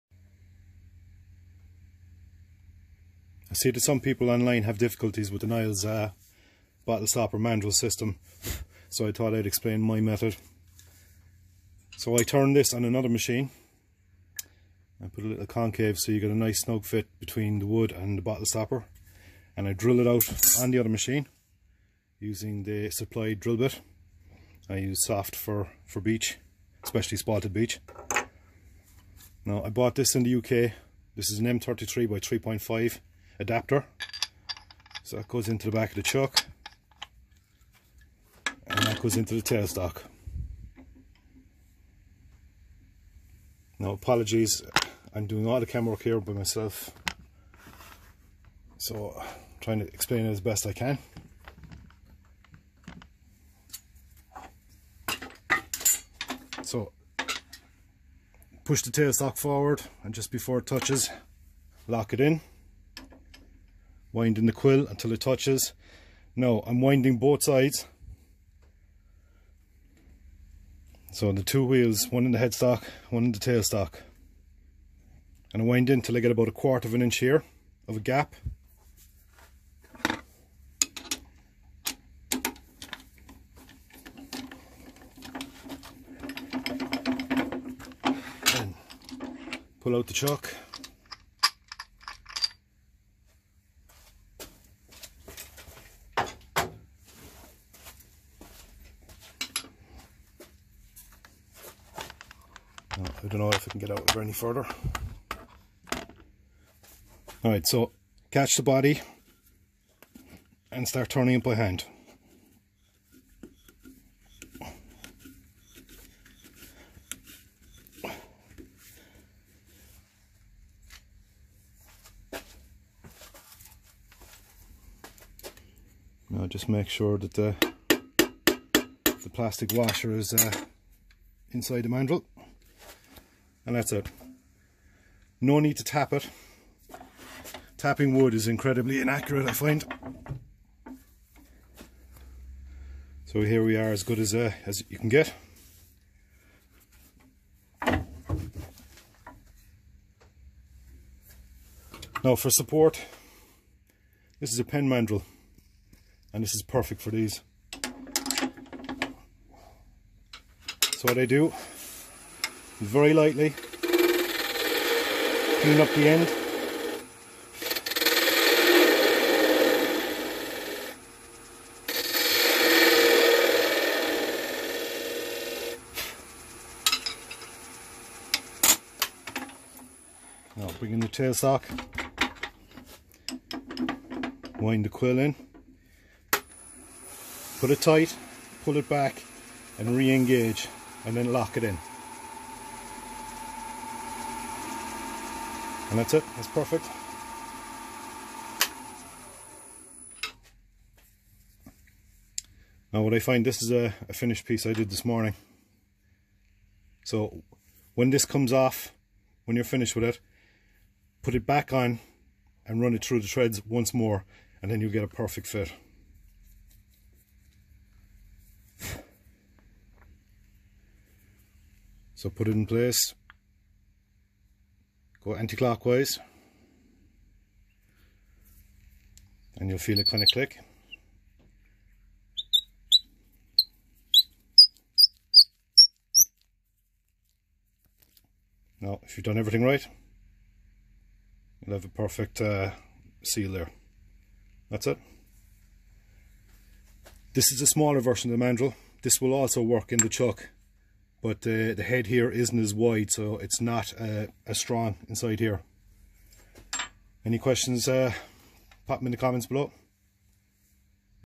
Using the Ruth Niles Bottlestopper Mandrel System
I have seen people uploading videos about the Niles Mandrel System since Ruth handed over the business to Carl and Robin Jacobson and I thought I would add my own method. Some comments I've seen suggested that some people had difficulties so I just wanted to show how I made it simpler for me.

When I first got the system I had a few issues tapping wood accurately and quickly. The results weren't great and on one occasion I lost the threads in the wood. I experimented and ended up using the system in this video. It is much quicker and the results are fairly consistent. I should mention that the Ruth Niles System is the best on the market that I have used.

I will explain the stopper fitting a bit better. I wind it anti-clockwise until the thread slips into place with a little jolt and then wind clockwise making sure I am correctly winding into the thread. If it starts to go wrong, I wind it back a bit and try again. This works for 99% of the bottlestoppers I make.

Apologies for the camera work, it's quite difficult on your own to talk and get the right picture in the frame.